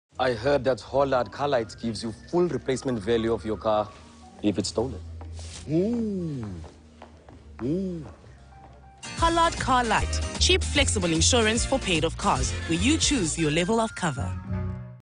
Hollard Car Lite – Replacement Sting
Car Lite pays out the full replacement value of your car if it is stolen. Join us in celebrating the launch of Hollard Car Lite. Cheap, flexible insurance for paid-off cars.